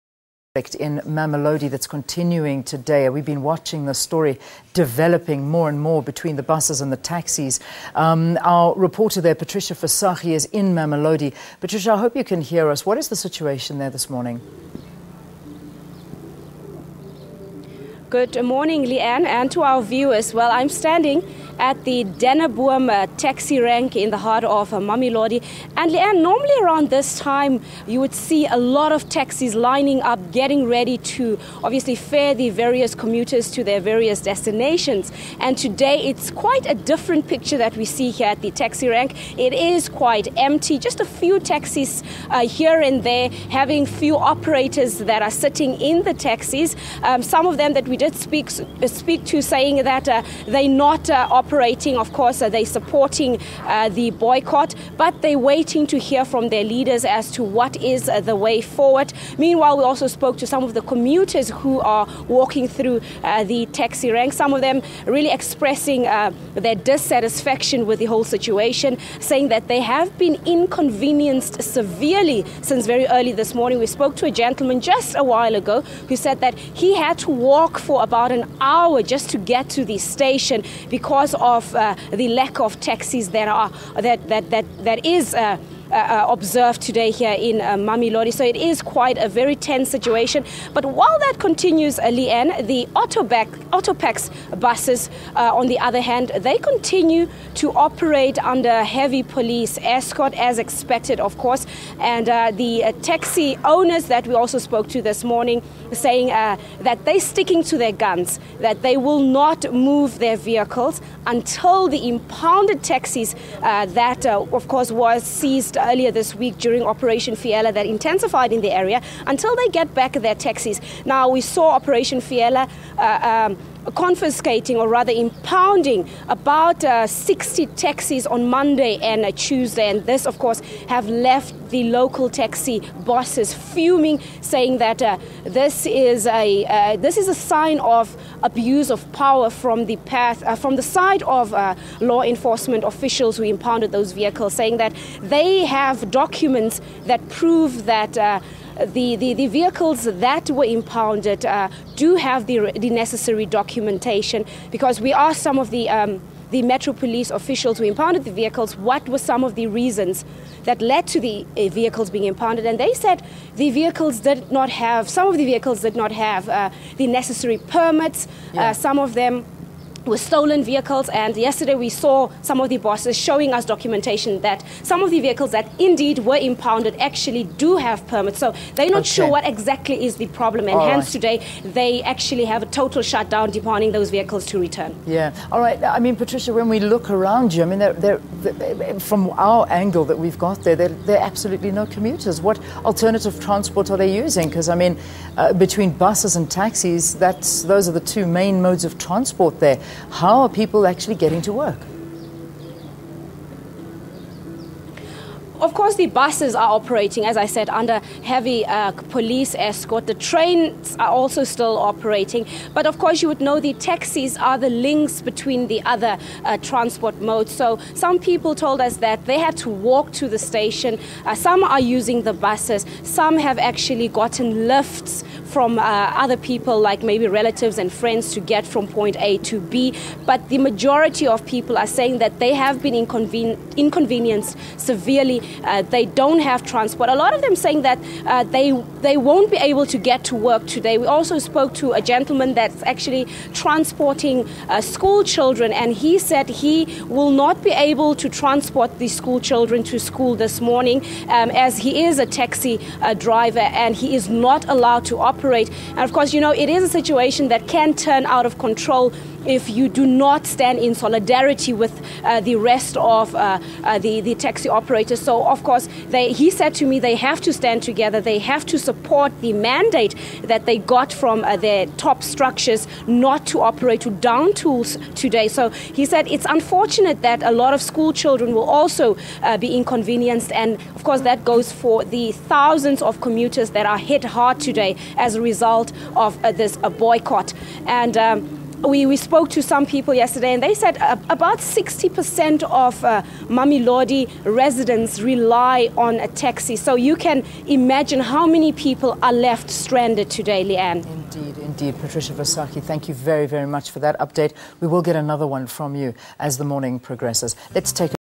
Taxis not operating in Mamelodi, Patricia Visagie reports
The ongoing transport problems and conflict in Mamelodi continue.  We cross live to our reporter Patricia Visagie in Mamelodi for an update.